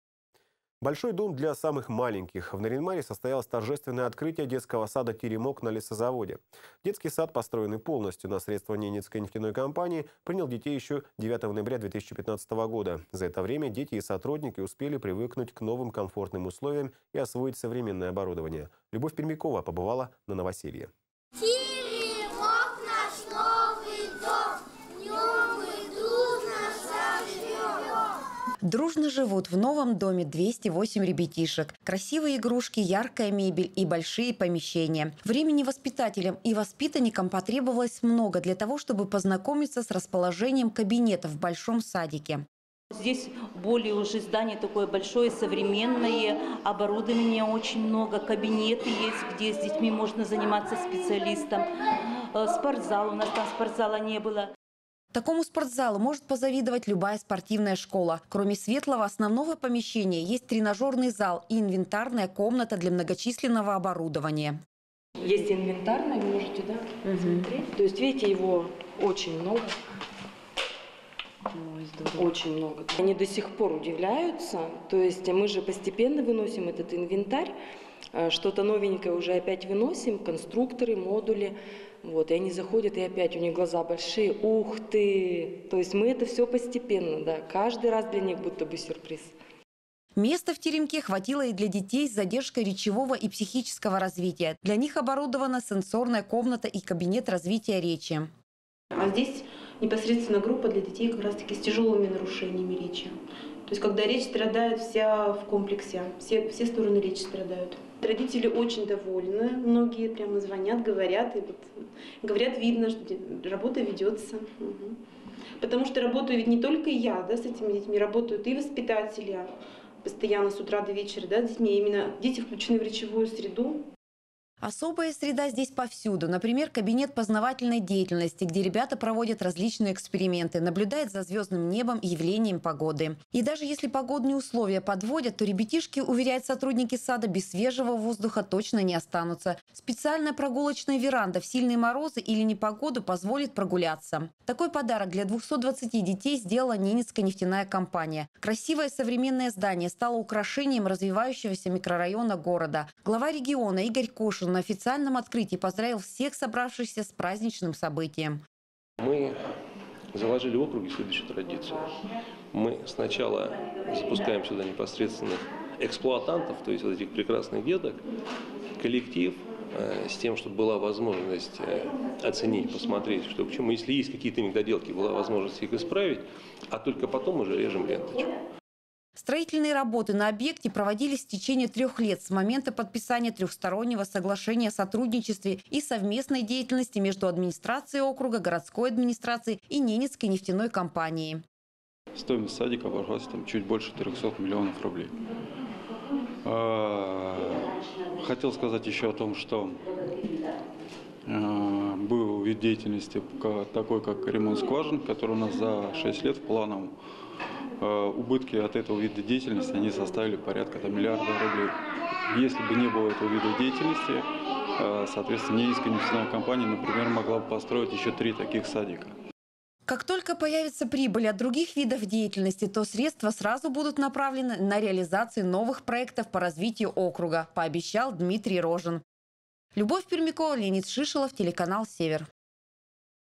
В Нарьян-Маре состоялось торжественное открытие детского сада «Теремок»
Большой дом для самых маленьких. В Нарьян-Маре состоялось торжественное открытие детского сада «Теремок» на Лесозаводе. Детский сад, построенный полностью на средства Ненецкой нефтяной компании, принял детей еще 9 ноября 2015, за это время дети и сотрудники успели привыкнуть к новым комфортным условиям и освоить современное оборудование. Корреспондент телеканала «Север» Любовь Пермякова побывала на новоселье.